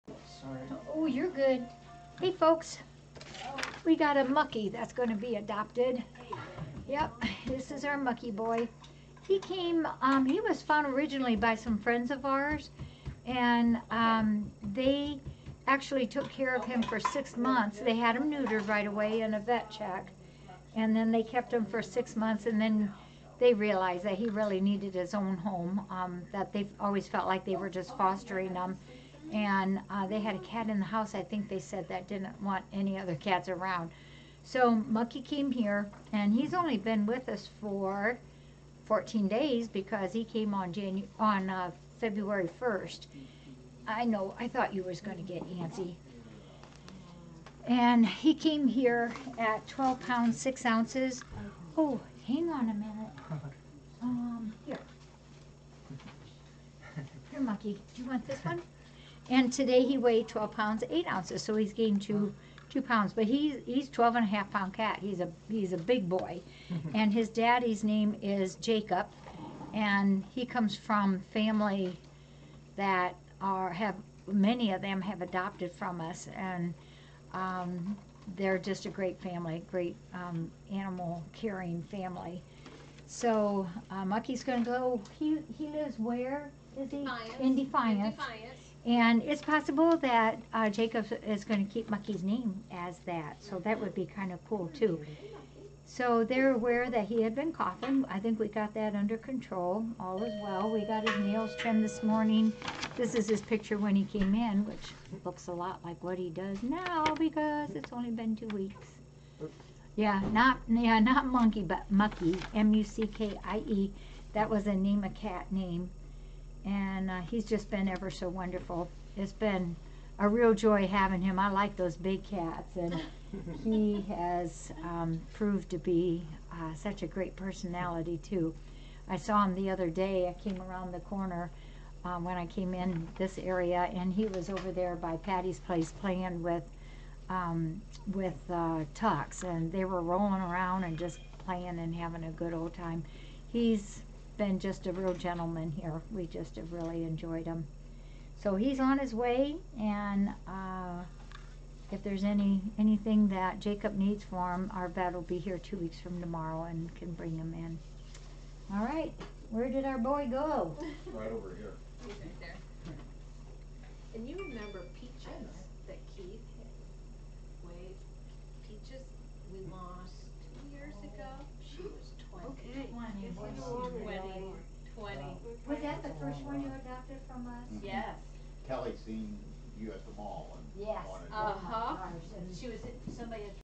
Adoption of Muckie 02/14/2020 Friends of Felines Rescue Center (FFRC)
Muckie finds his fur-ever home!!

Cage free, no kill, cat rescue who also cater for disabled cats.